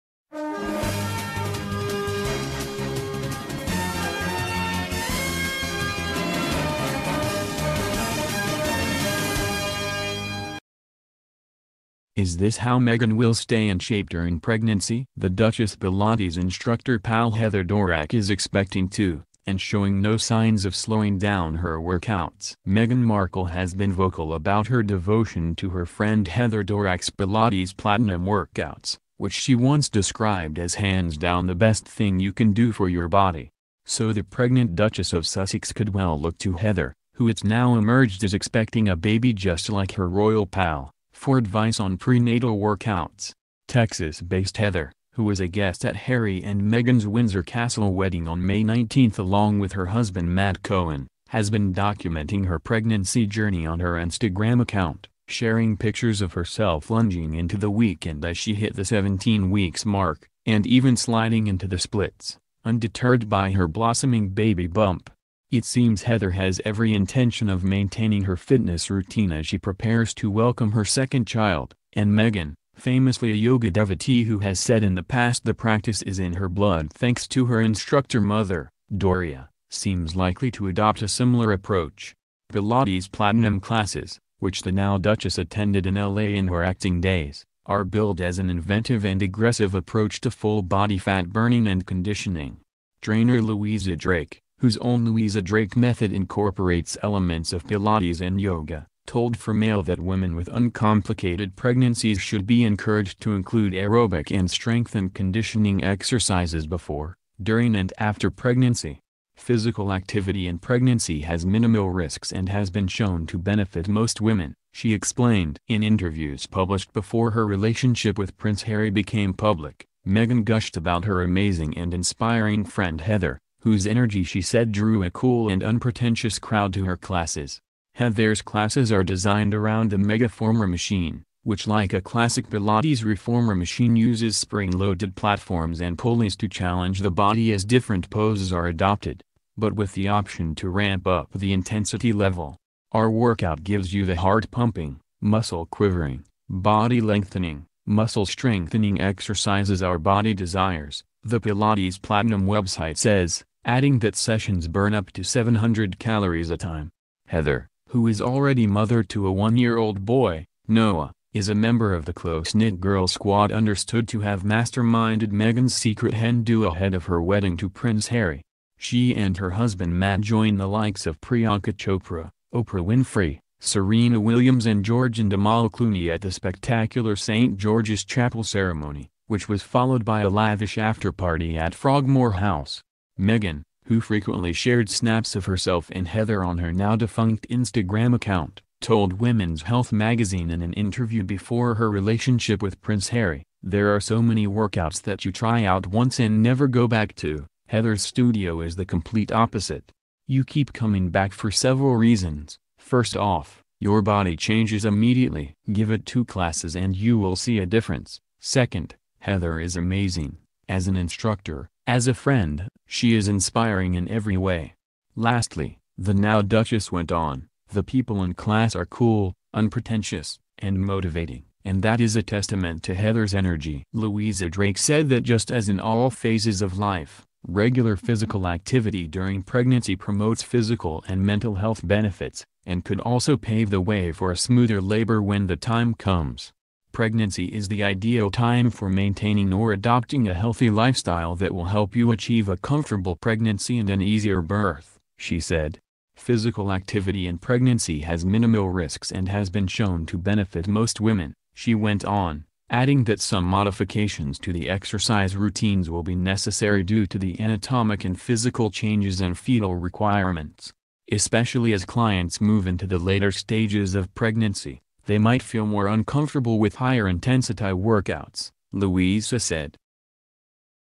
Meghan Markle's guide Heather Dorak reveals how she will stay in shape during pregnancy
Is THIS how Meghan will stay in shape during pregnancy. The Duchess’ pilates instructor pal Heather Dorak is expecting too – and showing no signs of slowing down her workouts.